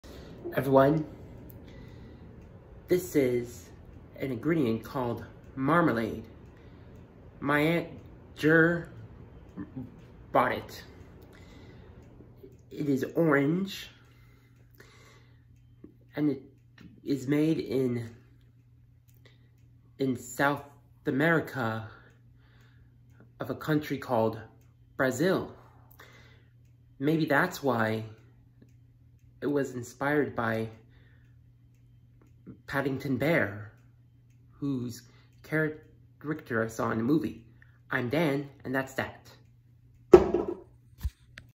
My Review on Marmalade
This is me reviewing a food ingredient called marmalade.